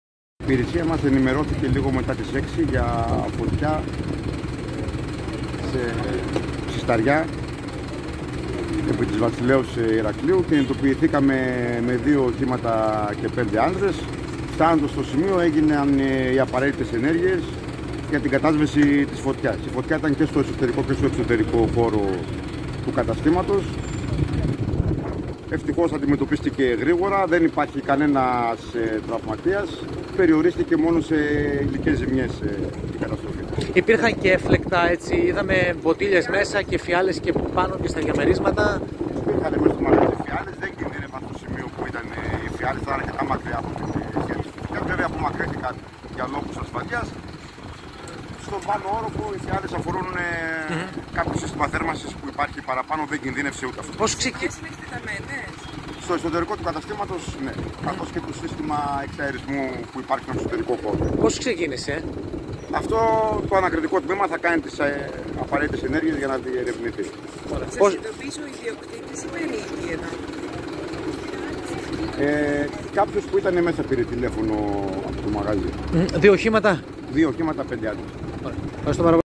Φωτιά σε κατάστημα εστίασης στην πόλη των Σερρών- Δηλώσεις Υποδιοικητή Π.Υ. (25/01/2024)
✏️  Διαβάστε το σχετικό ρεπορτάζ 📽 του Infonews24

📌 Ρεπορτάζ 🖥: Φωτιά ξέσπασε σήμερα το απόγευμα, σε κατάστημα εστίασης, στην πόλη των Σερρών και συγκεκριμένα επί της οδού Βασιλέως Ηρακλείου.
Στο σημείο επιχείρησαν δύο οχήματα της Πυροσβεστικής Υπηρεσίας Σερρών, με πέντε πυροσβέστες, ενώ παρών στο συμβάν ήταν και ο Υποδιοικητής της Π.Υ. Σερρών Παντελής Ελευθεριάδης.